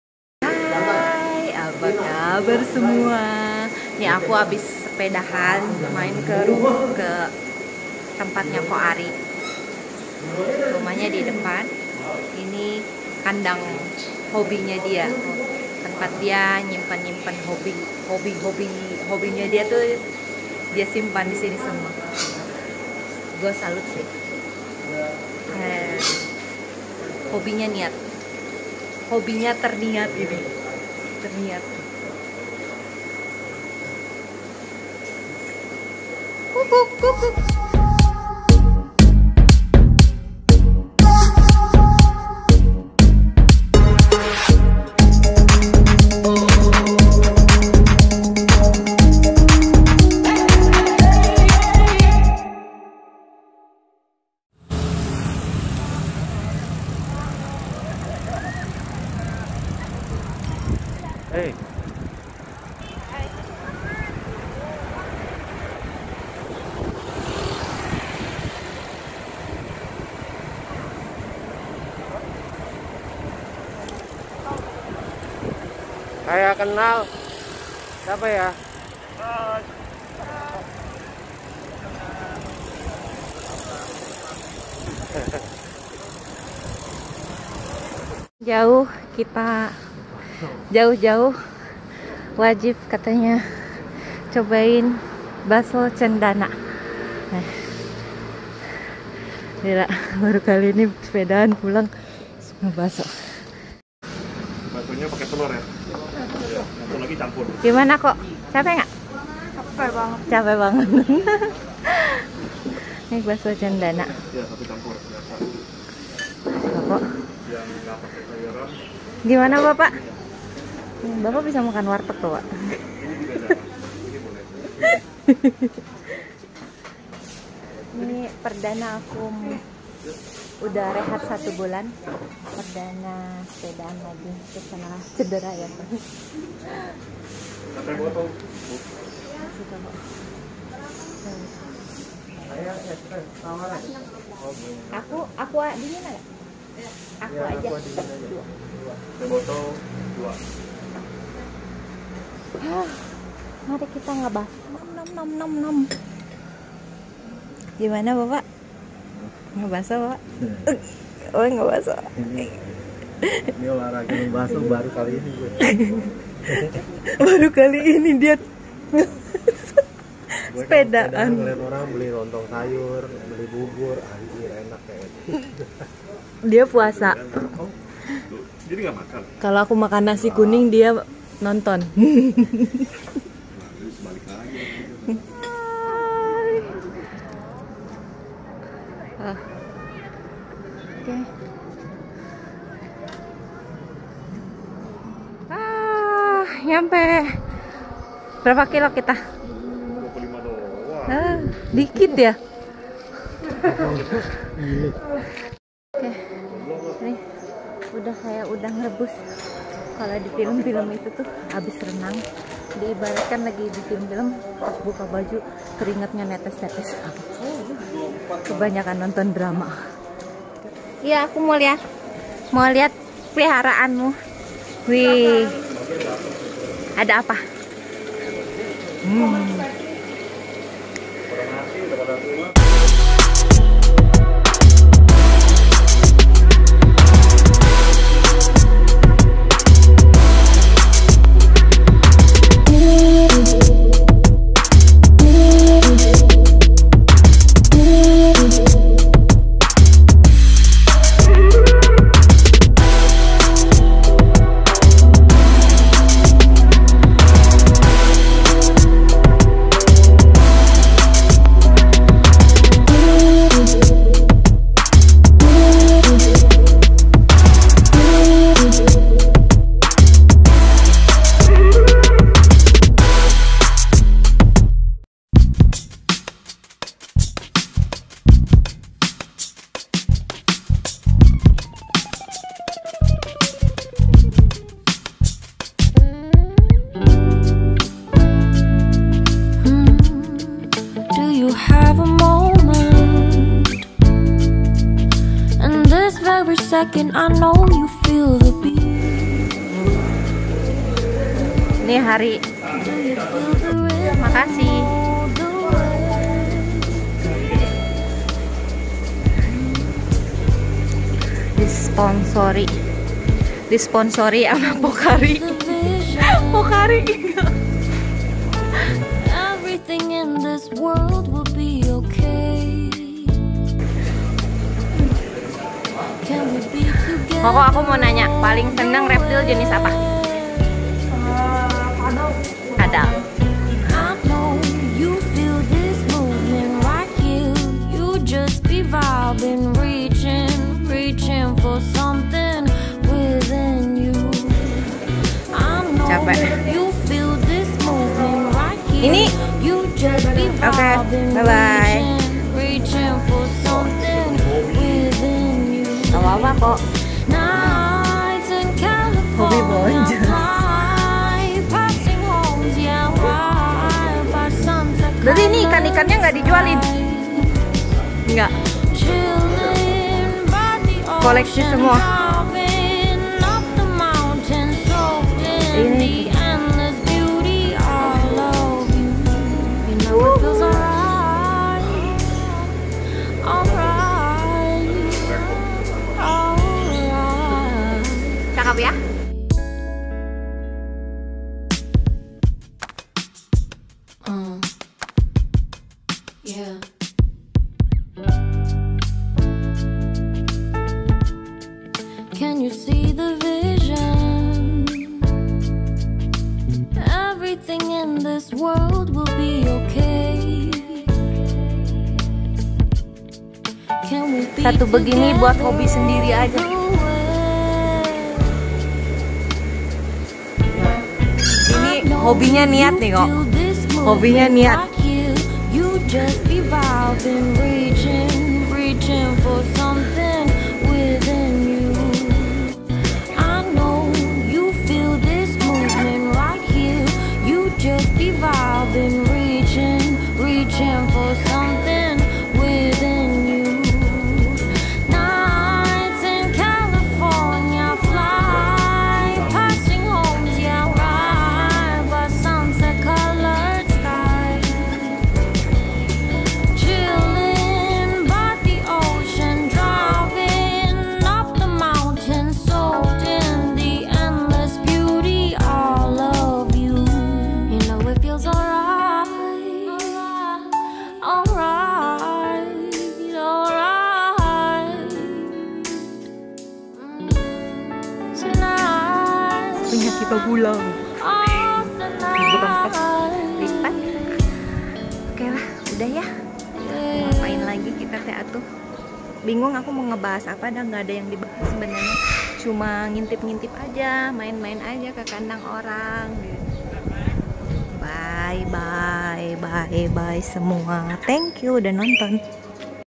Habis sepedaan | Lihat koleksi ikan & reptile ko Ary
Halloo sahabat gisku , selamat pagi, siang, sore, malam, semoga sahabat gisku sehat2 ya , dmanapun kalian berada

lokasi : jakarta

Pengambilan gambar ini saya ambil pada tanggal 21 maret 2021